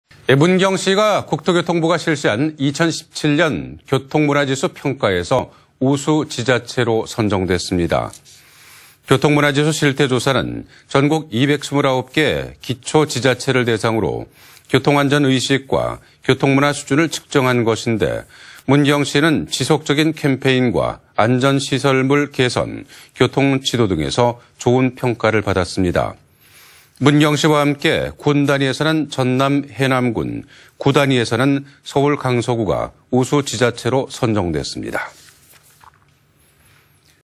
[안동MBC뉴스]문경 교통문화 '우수 지자체' 선정
2018/01/03 13:13:01    작성자 : 김건엽

문경시가 국토교통부가 실시한
2017년 교통문화지수 평가에서 우수 지자체로
◀ＥＮＤ▶

교통문화지수 실태조사는 전국229개
기초 지자체를 대상으로 교통안전의식과
교통문화수준을 측정한 것으로
문경시는 지속적인 캠페인과 안전시설물 개선,
교통지도 등에서 좋은 평가를 받았습니다.

문경시와 함께 군단위에서는 전남 해남군,
구 단위엣는 서울 강서구가 우수 지자체로
각각 선정됐습니다.